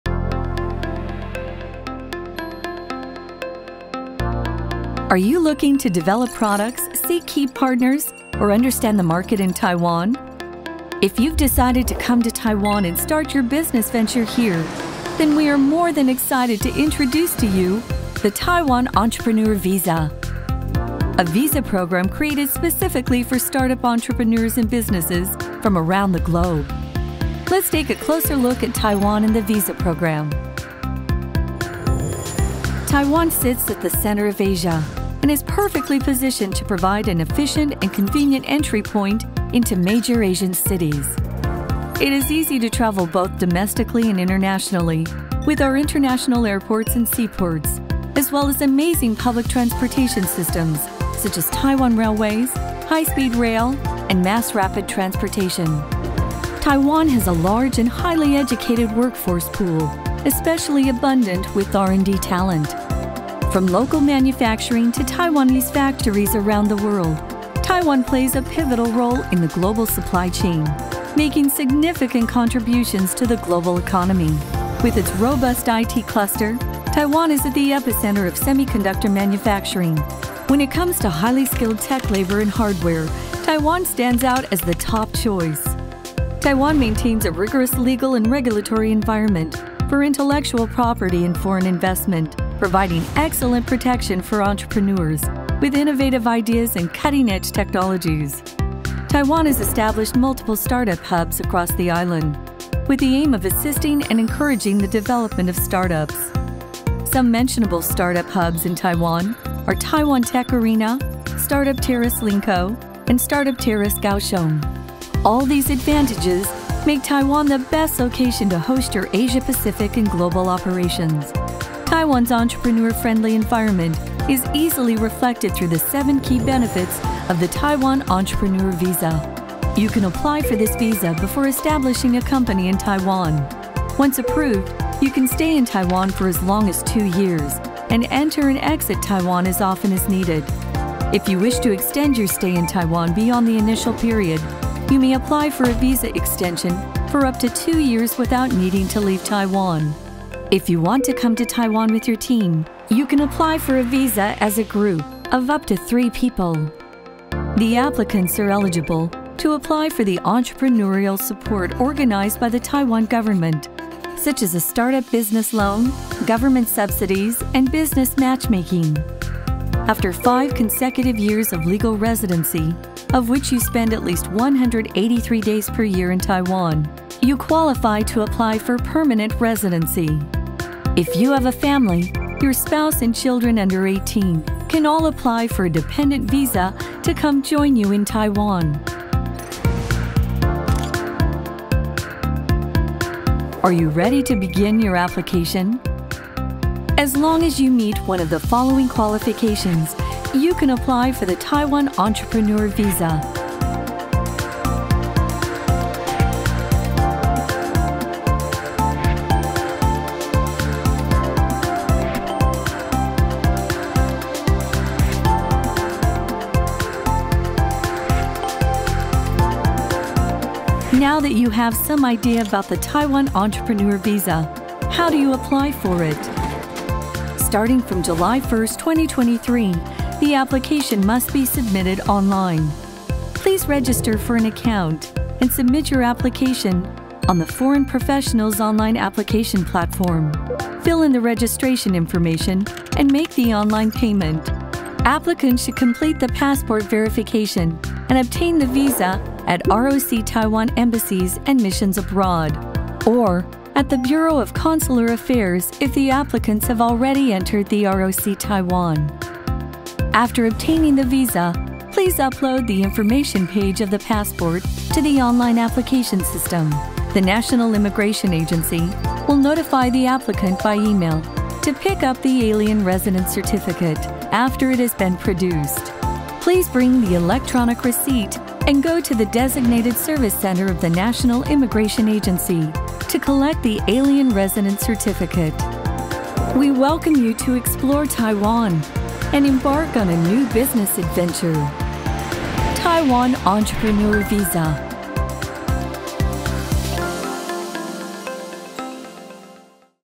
240712創業家簽證介紹影片
【Taiwan Entrepreneur Visa video】
Why do entrepreneurs choose to start a business in Taiwan?
What are the advantages of Taiwan Entrepreneur visa?
What are the eligibility criteria for applying for Taiwan Entrepreneur visa?
What is the application process for Taiwan Entrepreneur visa?
All the answers can be found in this 5-minute video.